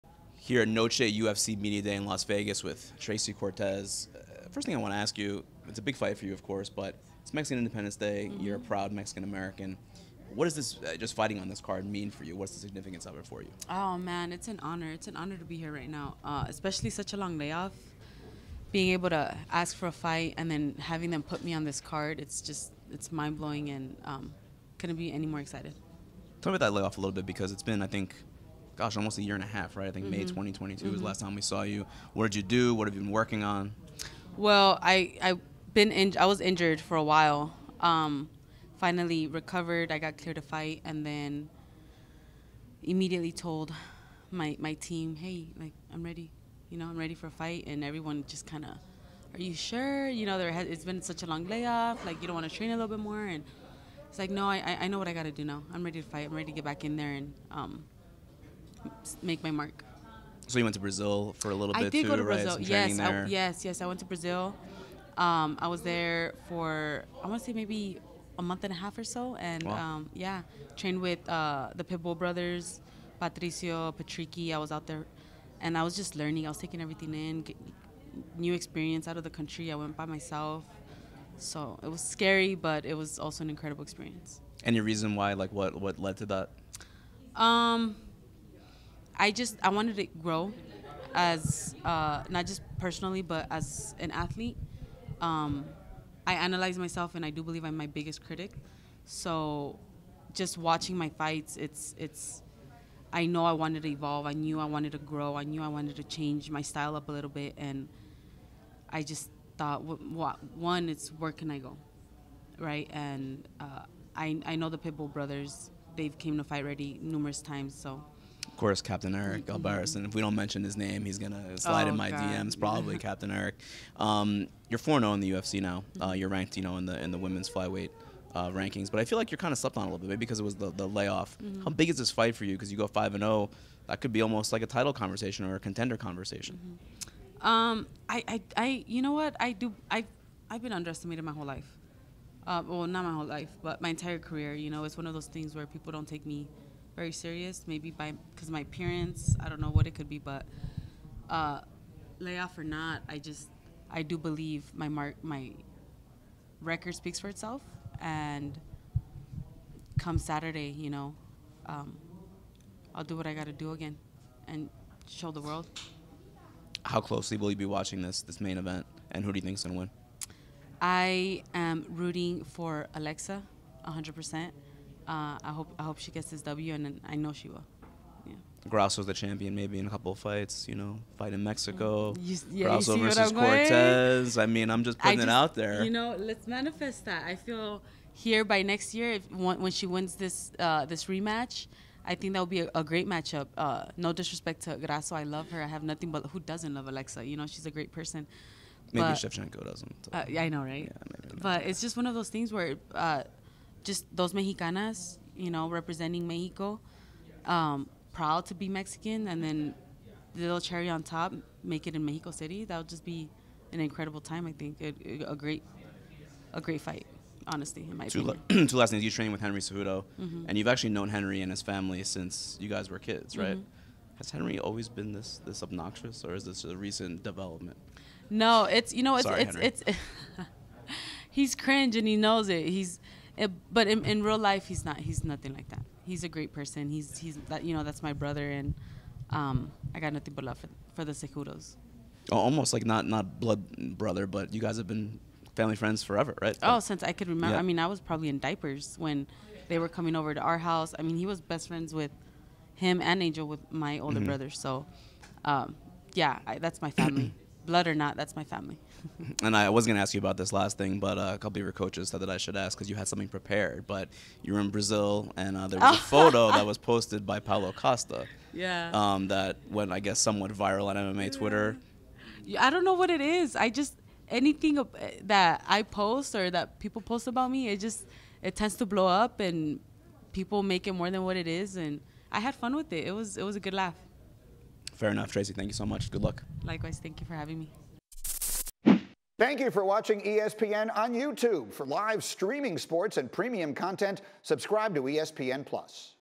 Tracy Cortez discusses how she’s evolved during her time away from the Octagon | Noche UFC
Tracy Cortez speaks with ESPN MMA’s Marc Raimondi at Noche UFC Media Day about her fight vs. Jasmine Jasudavicius, her fight since UFC 274 in May 2022. Cortez describes training in Brazil, how Henry Cejudo is like family to her and more.